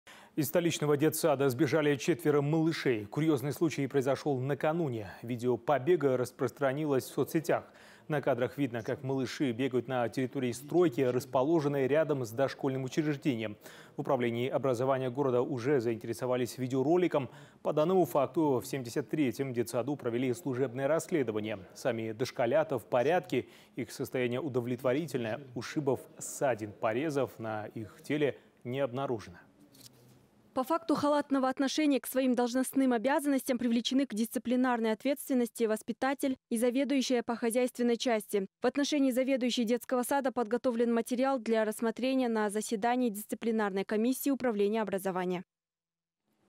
Четверо малышей сбежали из детсада на стройку в Нур-Султане
Из столичного детсада накануне сбежали четверо малышей, передает Almaty.tv.